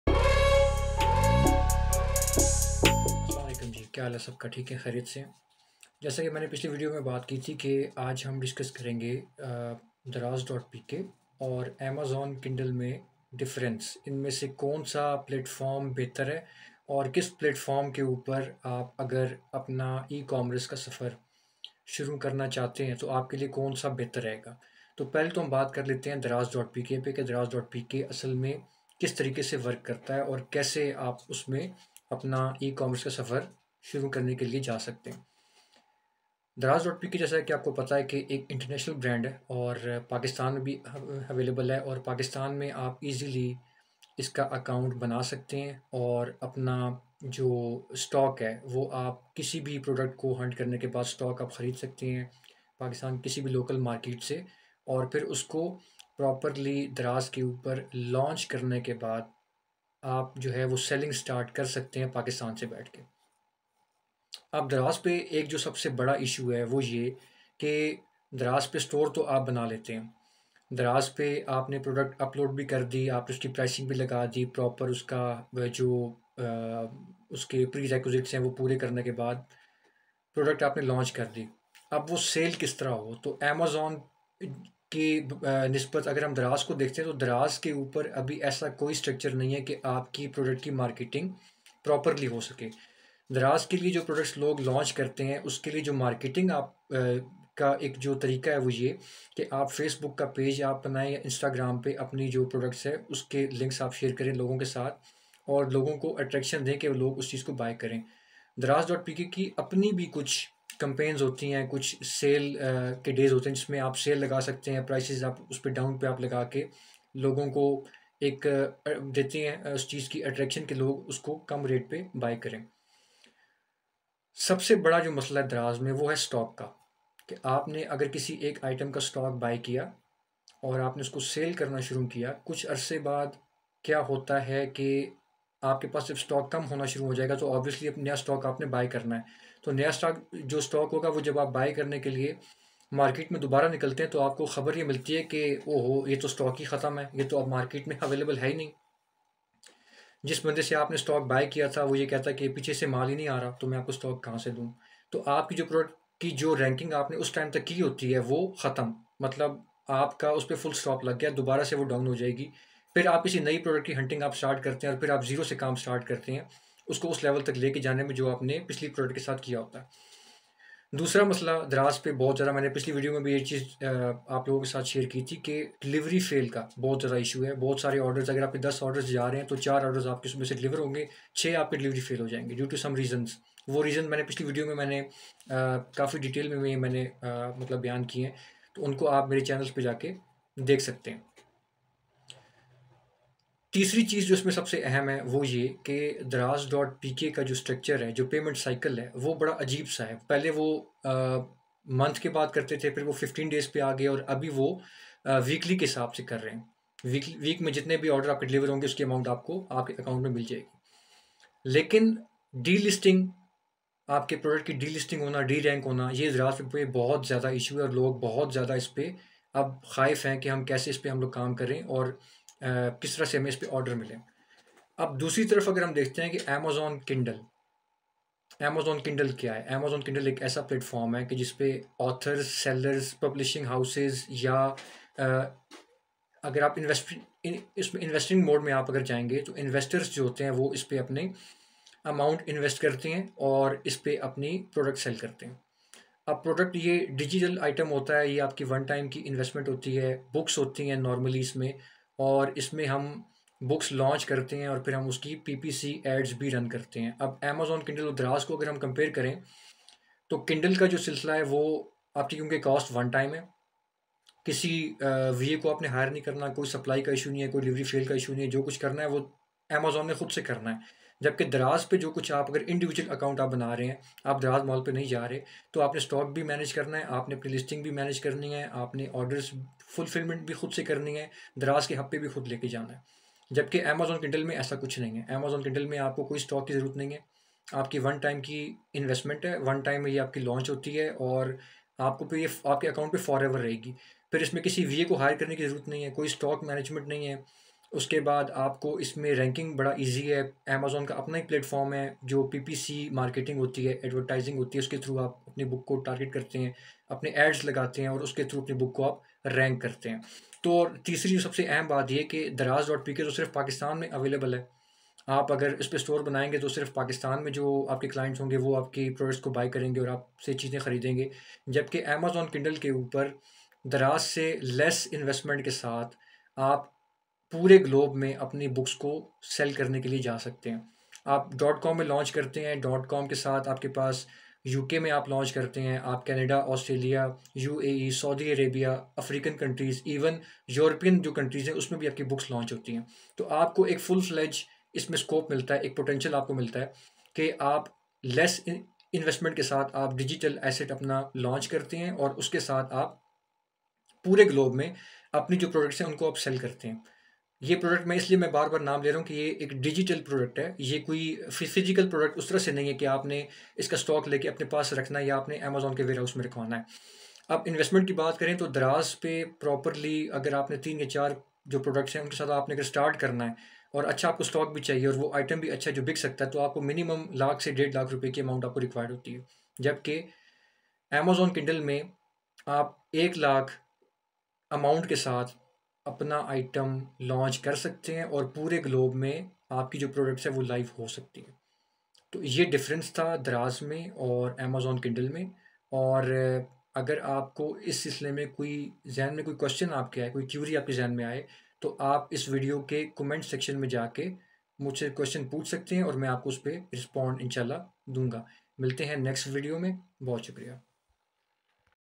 Daraz. pk vs Amazon Kindle KDP, A deep analysis and Comparison between them
Deep analysis in between Daraz and Amazon Kindle KDP Businesses and Pros and Cons of the platforms, investment and potential wise comparison. Which one is better to start with and why.
Watch this complete video before jumping in any of two.